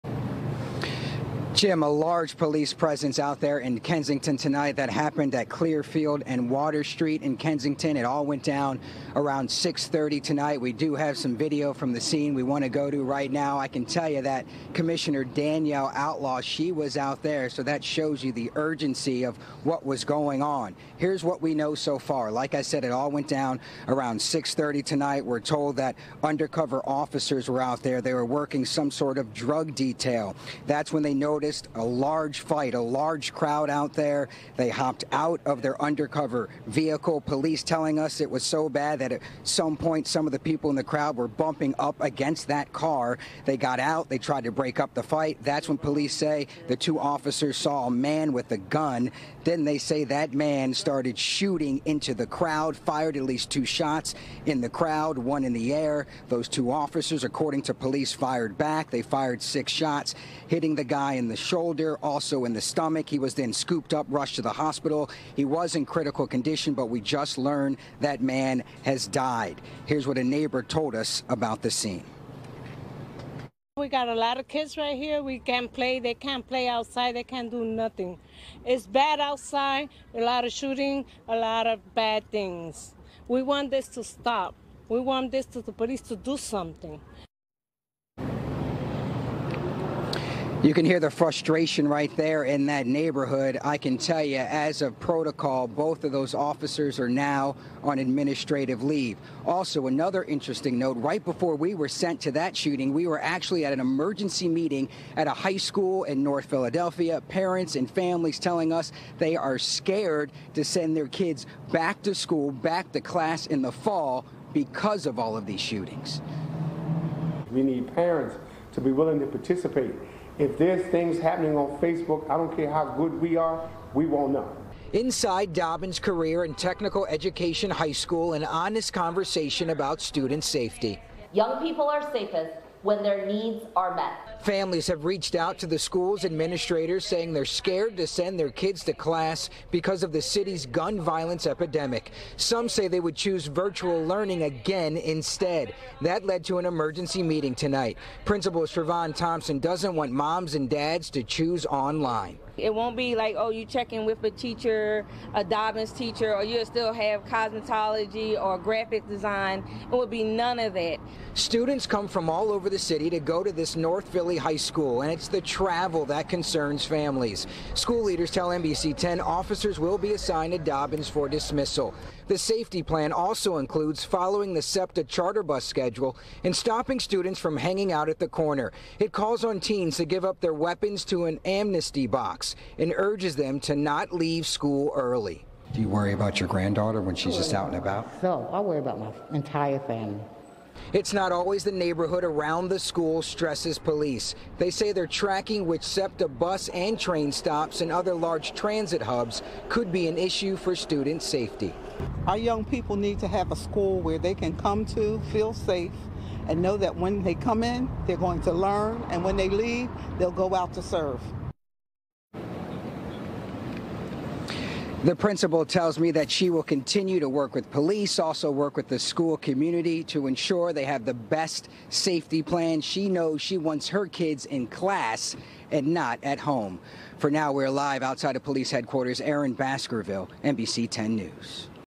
Officers Shoot and Kill Gunman Who Fired Into Crowd in Kensington, Police Say
Philadelphia police shot and killed a man who they say fired into a crowd during a large brawl in Kensington on Thursday. NBC10’s Aaron Baskerville has the latest details on the shooting. He also speaks with North Philly parents who say they fear sending their children back to class in the fall due to the surging gun violence.